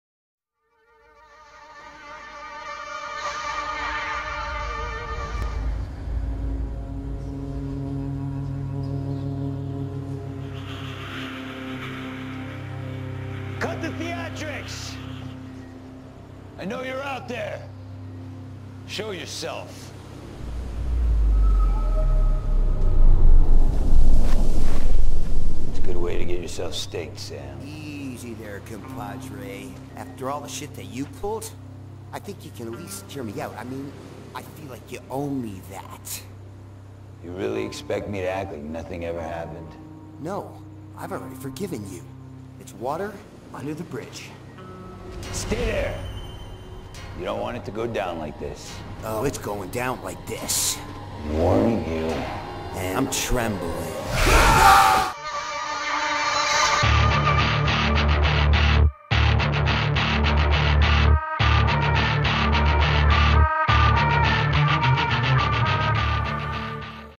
Thank You! (Lost Boys Fans)
I believe The Lost Boys Movie Trailer gets more comments on it than any other video I have uploaded. Thank you so much for all the feedback!